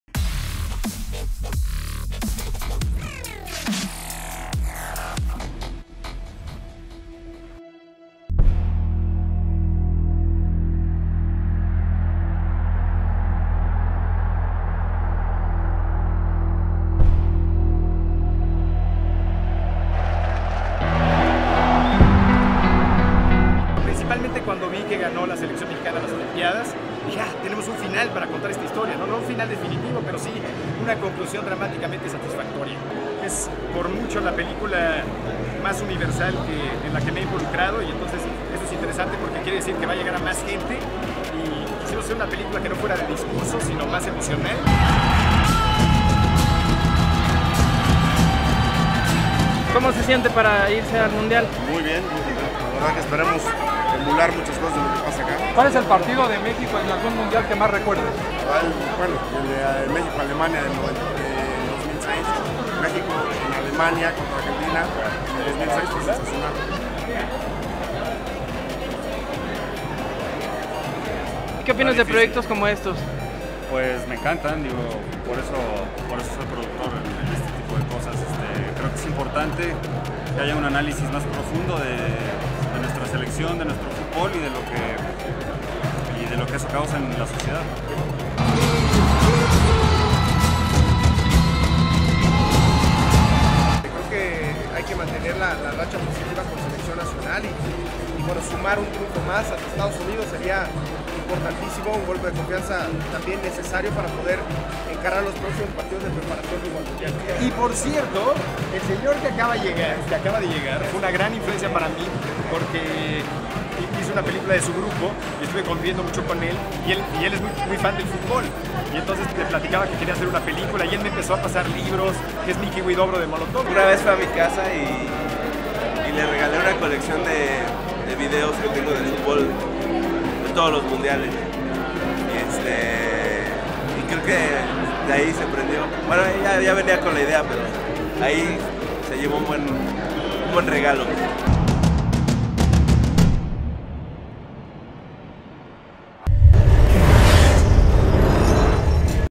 Alfombra roja de Ilusión Nacional
Estuvimos durante la alfombra roja de la nueva película documental de Olallo Rubio titulada Ilusión Nacional.

El documental aborda la historia de la Selección Mexicana de Futbol a través de los mundiales y torneos oficiales de la FIFA. Durante la alfombra roja pudimos platicar con el director y demás invitados que asistieron al evento, entre los que se encontraban Micky Huidobro de Molotov, José María Yazpik productor de la película, Miguel Herrera DT de la Selección Mexicana y Moisés Muñóz, portero del América.

Ilusión Nacional se estrena este viernes 4 de abril de 2014, ¿fans del futbol por aquí?